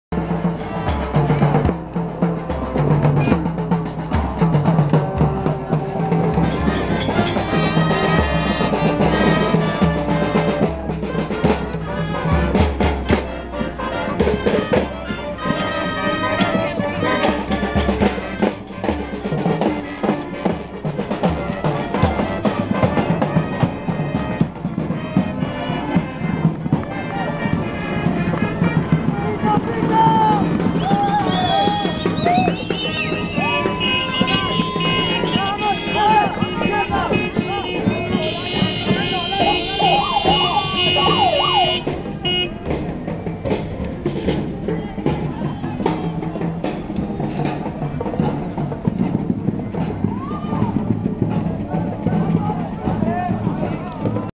El hombre más alto del Festival Iberoamericano de Teatro de Bogotá
El hombre más alto del festival es un maniquí de hilos gigante. Saluda a la gente que espera a que el desfile pase para poder continuar con su camino. La gente acompaña el ambiente festivo con los pitos de sus vehículos.